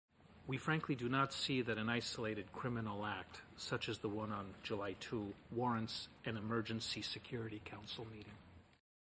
Ambassador Wolff Remarks on Situation in Kosovo
Deputy U.S. Permanent Representative to the United Nations, Ambassador Alejandro D. Wolff, remarks at a United Nations Security Council Session on the Situation in Kosovo, New York, NY July 7, 2010. [Go for text transcript and more video.]